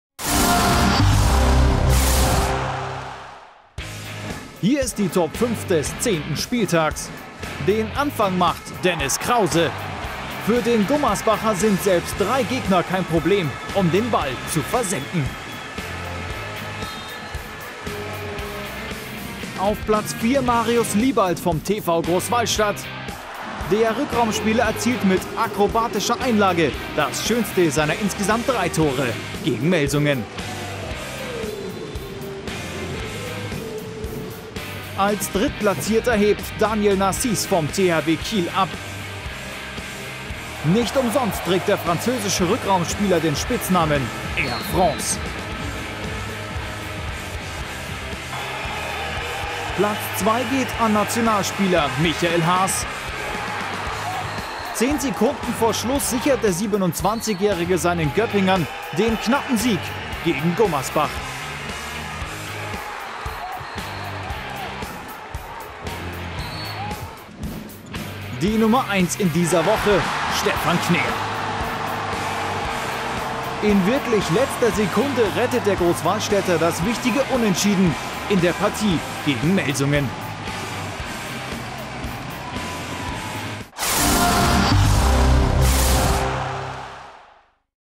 TOP5 DES 10. SPIELTAGS -SPORT1
Wir haben für sie die fünf schönsten Tore des 10. Spieltags der Toyota Handball Bundesliga!